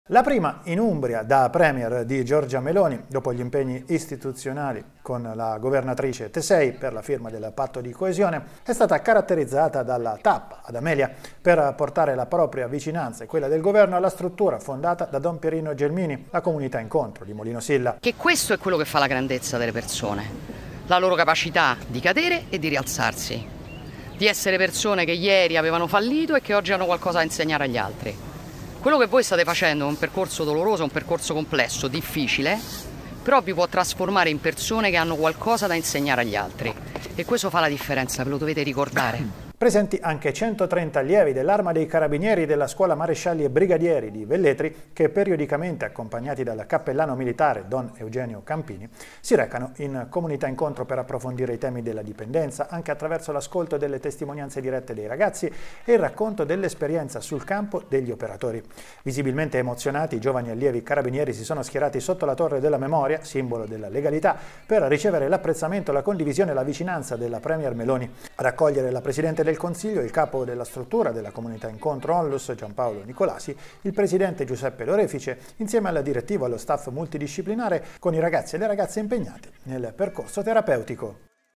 Comunità Incontro di Amelia: la visita di Giorgia Meloni
Di ritorno da Bastia Umbra, dove insieme alla governatrice Donatella Tesei ha sottoscritto l'accordo per lo sviluppo e la coesione tra Governo e Regione, la presidente del Consiglio, Giorgia Meloni, ha visitato la Comunità Incontro di Amelia. Una visita effettuata con l'intento di portare la sua vicinanza alla struttura fondata molti anni fa da Don Pierino Gelmini.

Servizio di Domenico Cantarini.